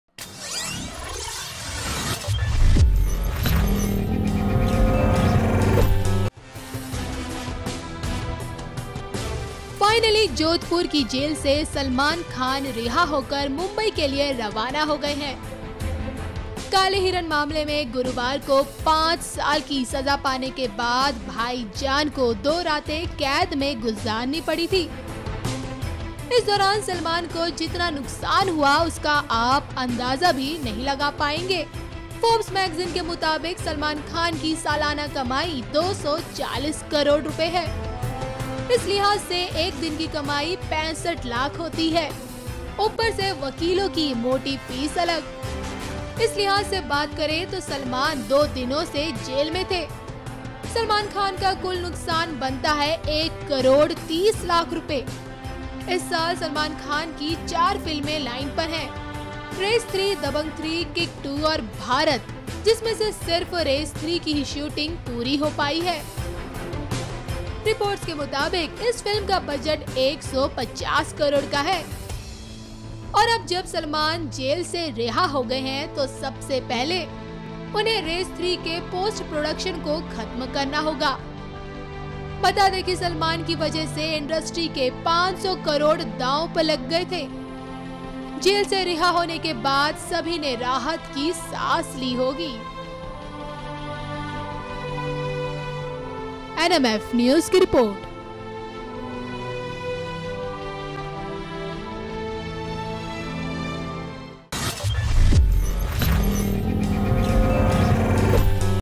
OMG! Salman के दो दिनों में डूब गए इतने Crore की जानकर चौंक जाएंगे आप
Salman Khan gets bail in the Blackbuck poaching case. Did You Salman Khan Lost 1 Crore 30 Lakh Rupees In Two Nights.to know more watch this video

Edit:-Pardeep Tomar
Producer:-prashant